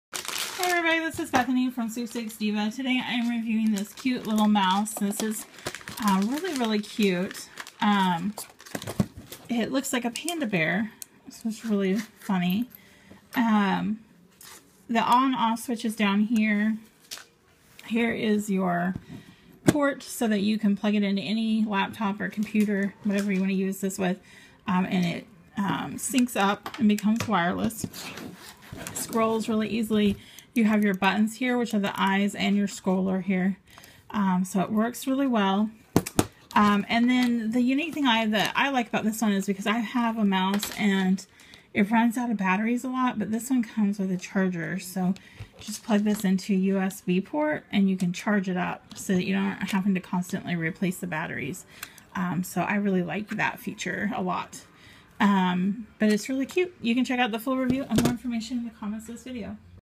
Sungwoo Wireless Panda Mouse
I just got this really fun panda wireless mouse from Sungwoo. This is a traditional wireless moue but it is designed to look like a panda bear. It's super cute, pink & black. It even has little ears. It still functions just like any other wireless mouse. It comes with the adapter to plug into your computer or laptop so that it is wireless. It comes with a charging cord that you can plug into the mouse to charge it up. I really like this feature. I have used wireless mouses in the past and they usually run on batteries and they go through them pretty fast usually. I like that I can plug this one in and run it off of a charge instead of batteries. It lasted a long time too I just charge it up over night and then use it during the day. Works great. Super cute, I love it! I received this item complimentary or at a reduced price for my honest review. However, I only recommend products I truly love and use to my readers.